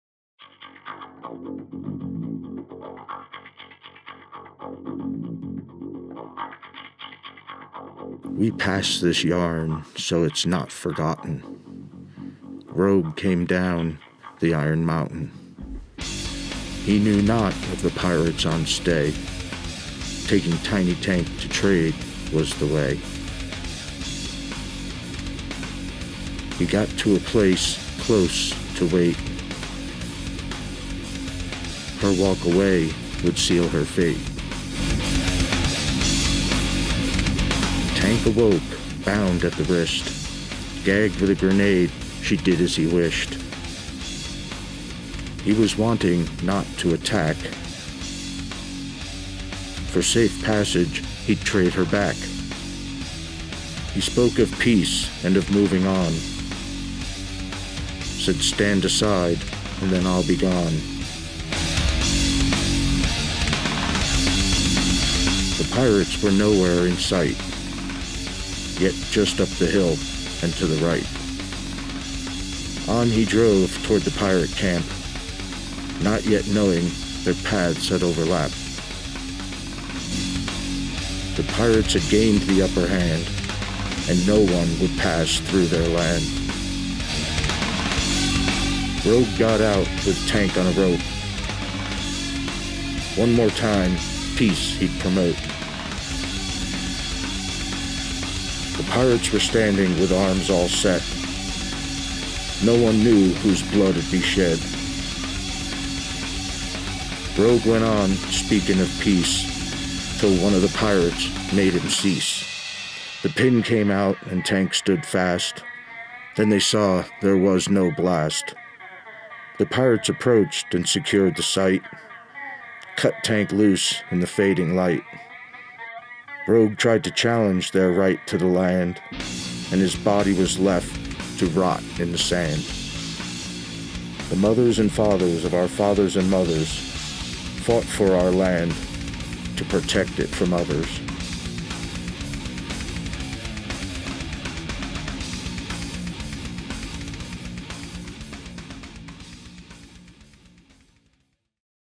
Safe Passage: an I phone film
We accepted the challenge to shoot a film for the Phone it in Film Festival. The rules are quite simple. You must shoot the film on a phone. It can be no more than 3 minutes total run time. Seems easy right? Well it was a bigger challenge than I expected. We were privileged to be able to shoot in a place conducive  to our Post Apocalyptic theme and we spent about 6 hours there shooting. Not all of it followed the plan. We made up a lot on the fly, but in the end we told the story of Rogue and how he met the Iron Mountain Pirates.